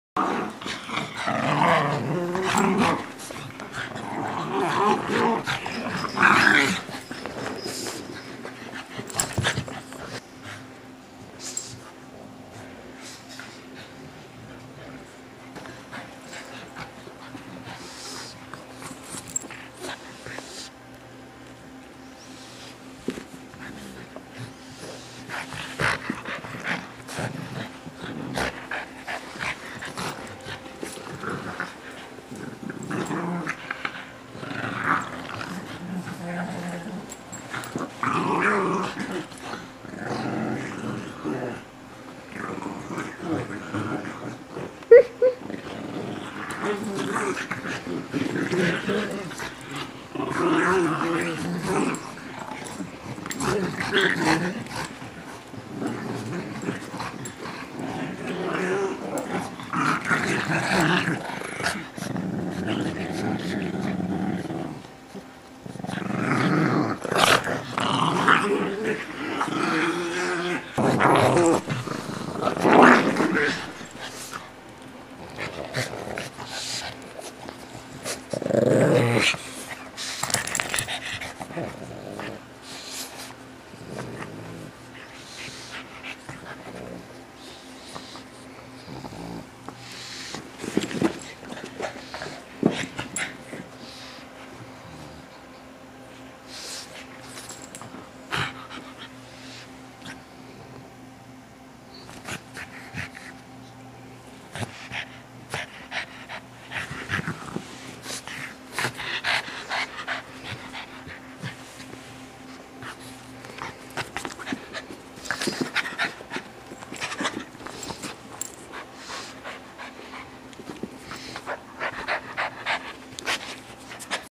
2004 Jaxon and Pooka Pug-O-War
Jaxon and Pooka playing tug-o-war with a monkey rope toy we bought them.